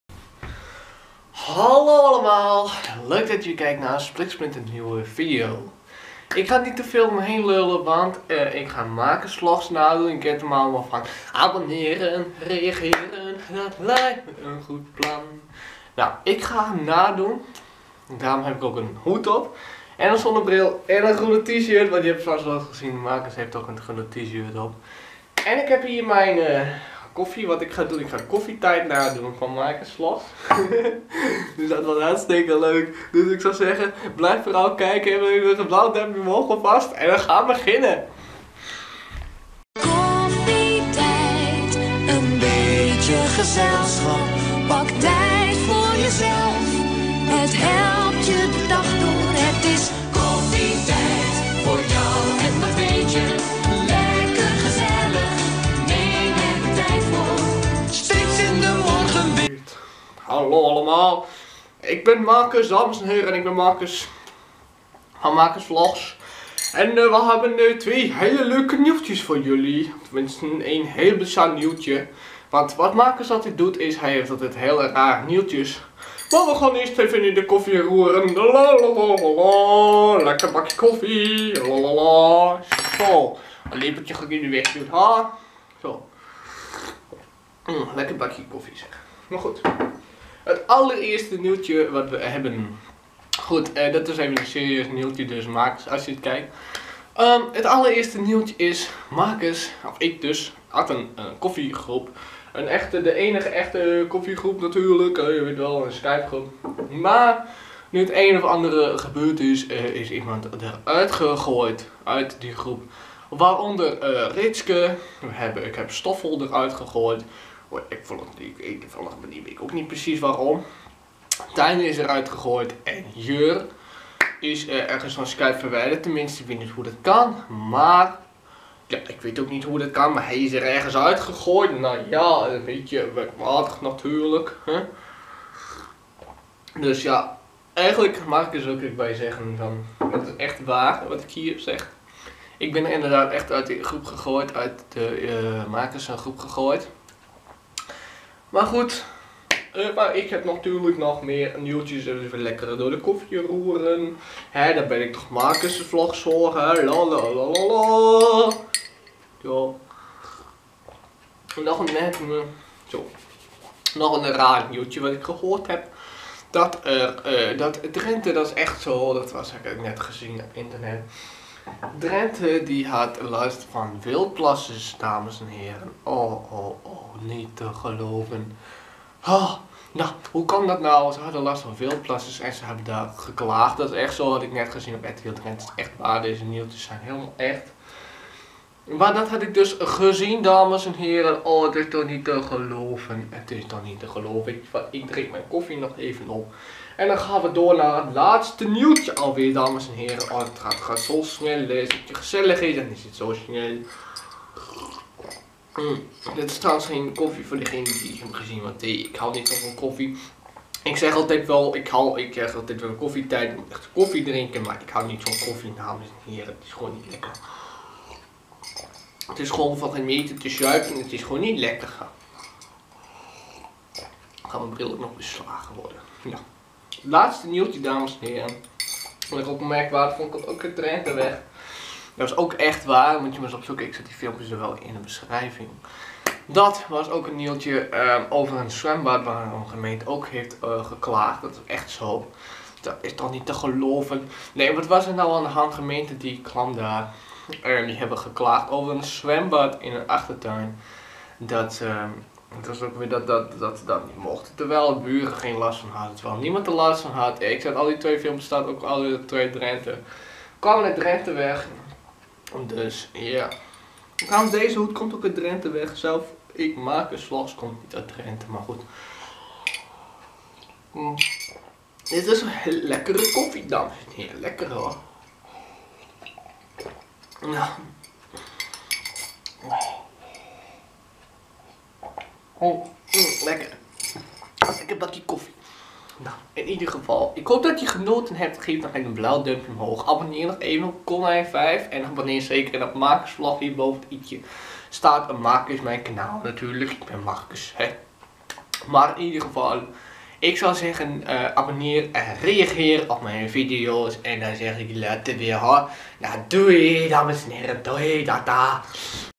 marcus vlogs koffietijd nadoen uit skype groep gezet konijn5
hallo allemaal vandaag doe ik marcus na voor de grap alle nieuwes die vertel ik wel echt waar ik ben echt uit de groep gezet en moet er nog in voor de rest hier zijn de twee video waar het het veder over had  veder is dit natuurlijk een grap dus niet  serieus nemen